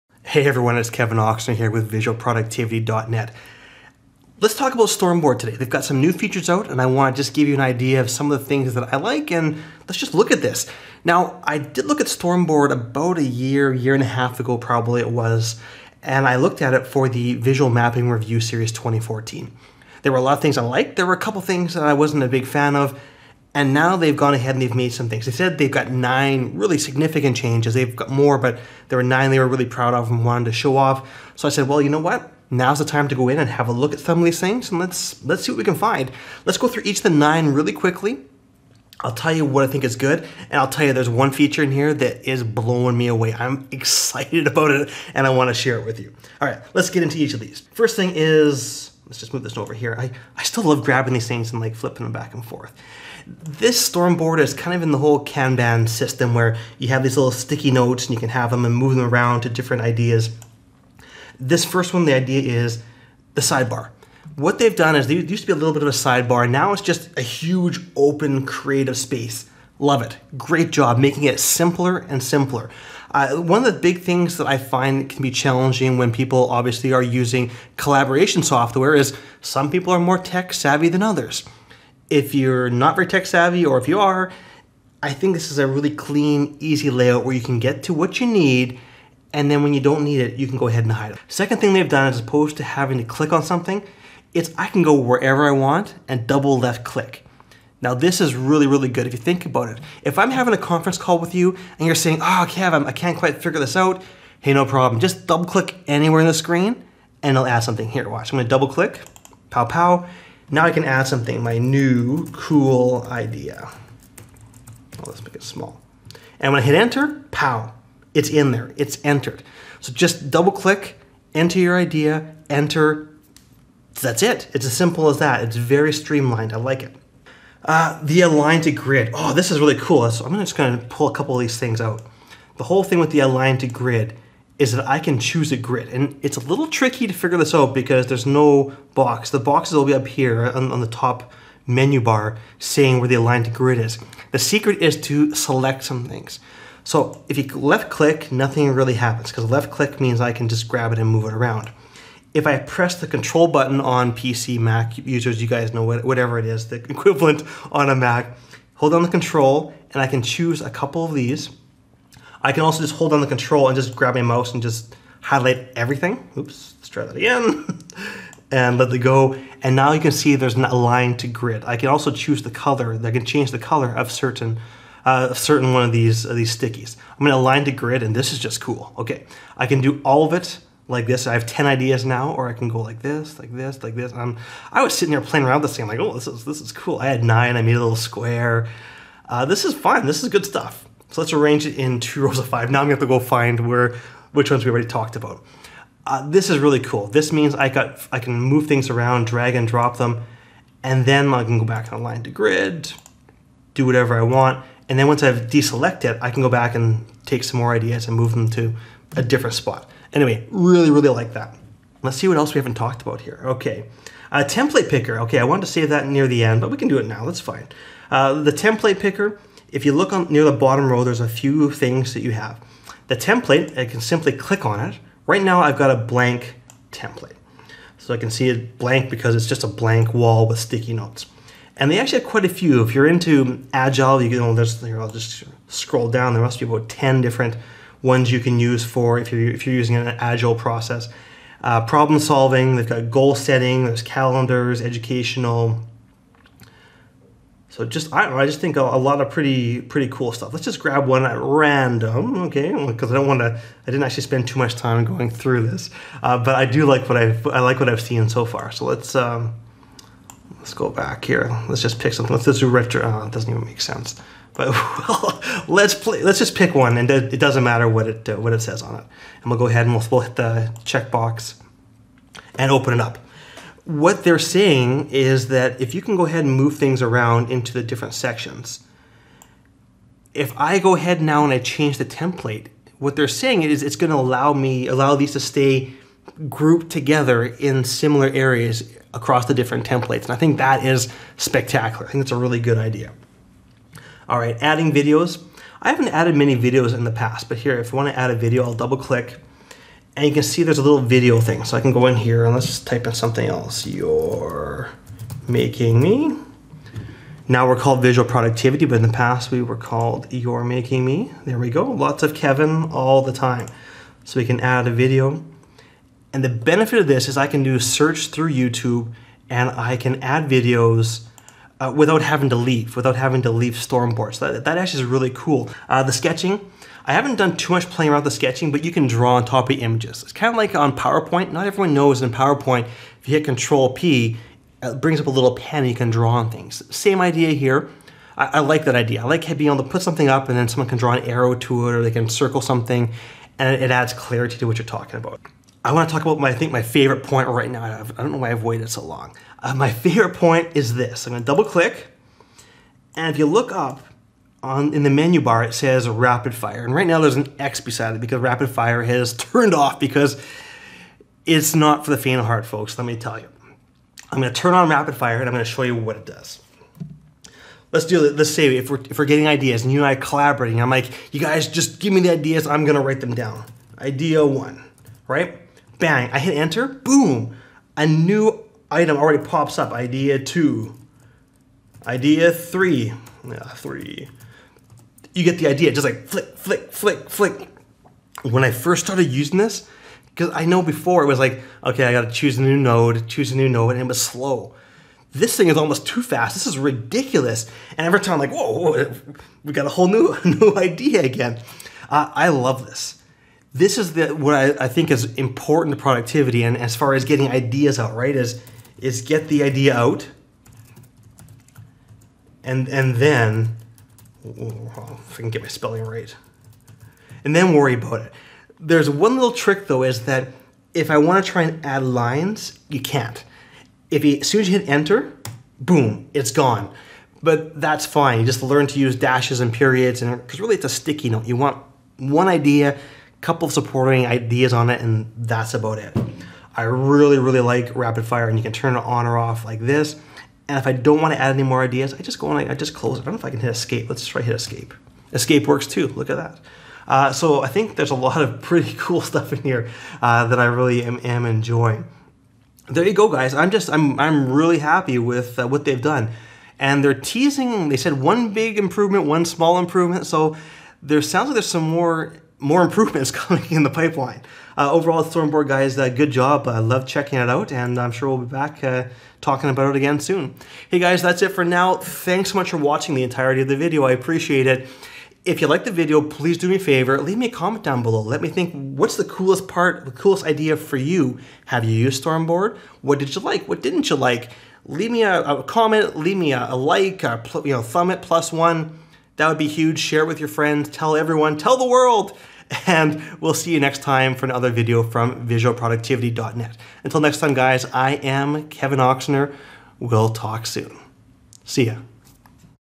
Stormboard Features
In this video I go over some of the new Stormboard features.

There are a lot of great changes. You really need to try out the rapid fire mode for yourself. It's great.

You can go over the complete blog post on my